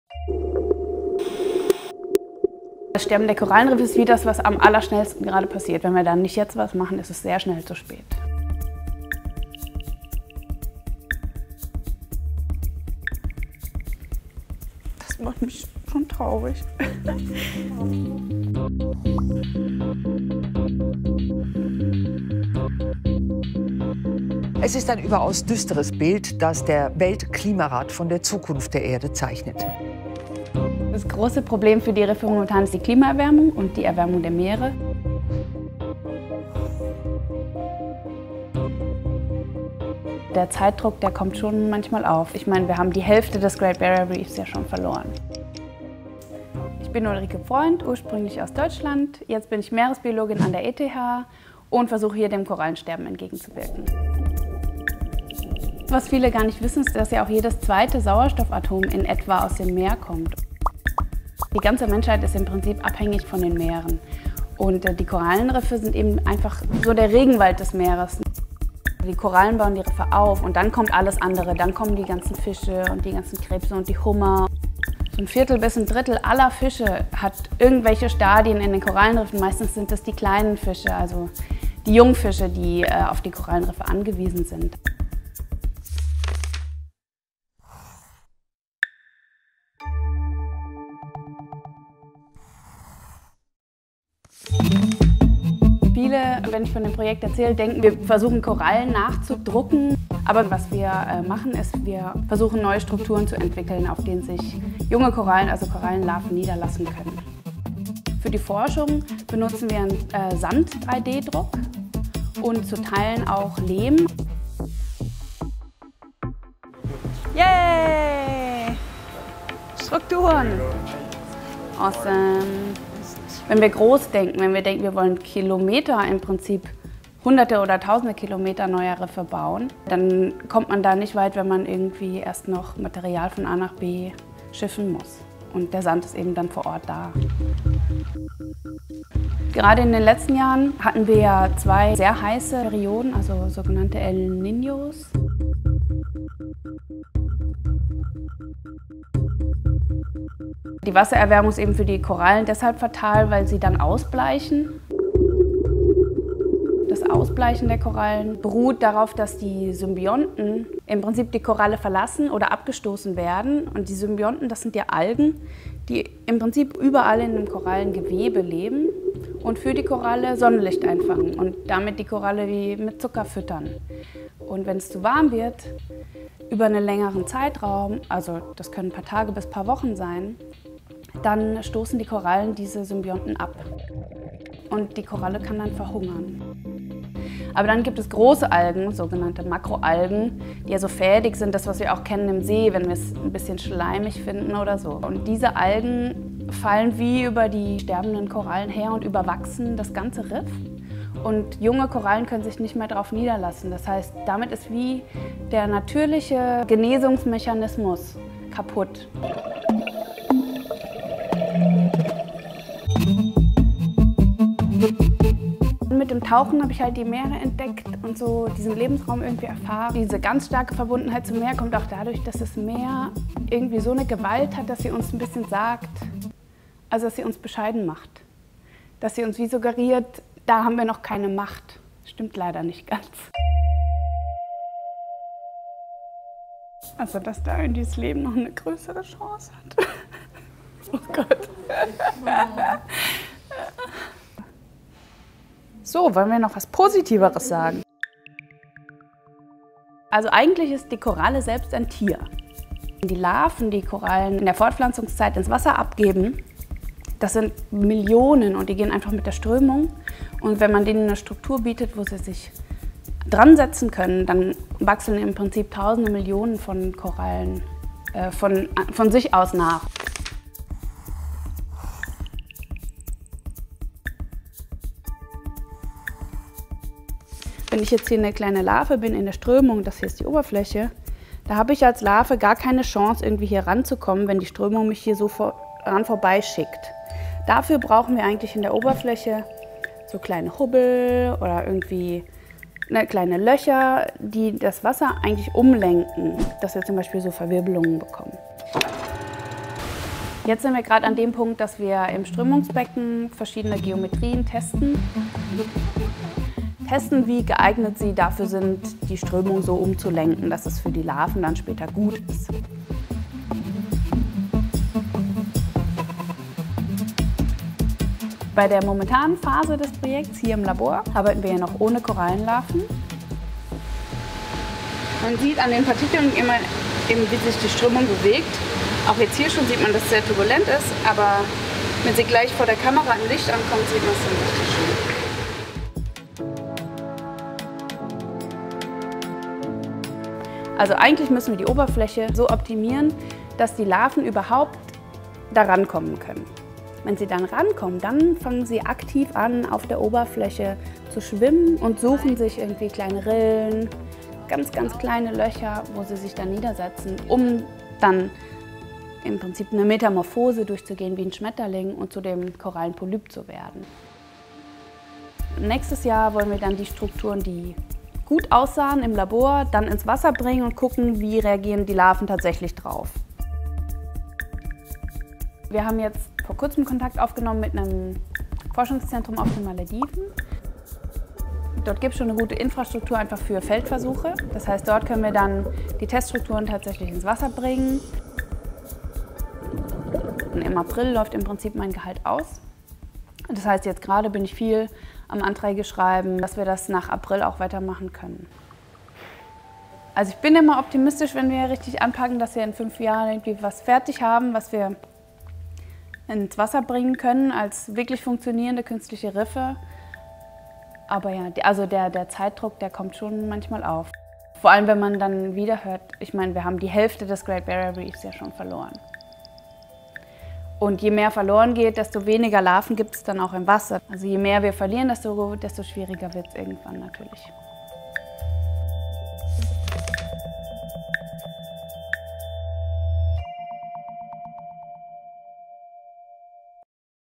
Can 3D printing save the coral reefs?
The oceans produce one in every two oxygen atoms. Coral reefs are like the ocean's rainforest. But it's disappearing, and fast. Ulrike Pfreundt has made it her life's work to find a solution.

Pfreundt is a marine biologist at the Federal Institute of Technology ETH Zurich who gets emotional when she talks about coral death. Through 3D printing, she's found a way to make artificial reefs and replace some of the coral that's being lost by providing structures for new coral to grow.

She's now testing the structures in the lab to see how to get coral larvae to start growing on them. Ultimately, she hopes to be able to deploy her printed inventions in the ocean to help grow new coral and support the vast ecosystems that depend on it.